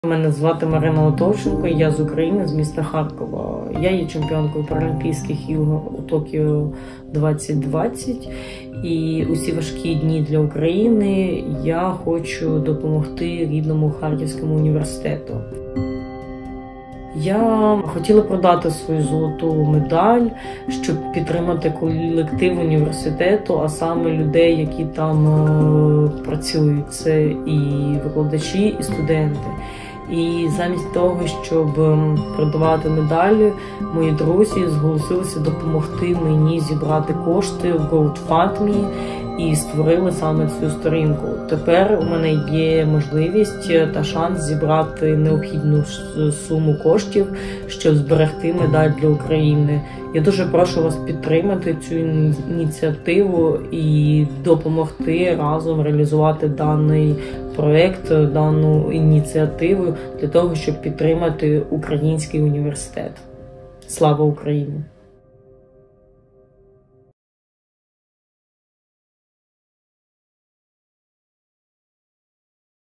Help Paralympian to support Ukrainian University!
My name is Maryna Lytovchenko.
I'm a professional parasport table tennis player. 🏓
I won a Gold Medal at the 2020 Paralympic Games in Tokyo.
I achieved victory because of the support of Karazin University in Kharkiv, among other things. Now part of the university buildings have been destroyed by Russian bombs.
I believe that now it is my turn to support university.
My goal is to raise $30,000 otherwise I will sell my medal.
Let's do it together! Everything will be UKRAINE! 💙💛 (fundraising link in the first comment)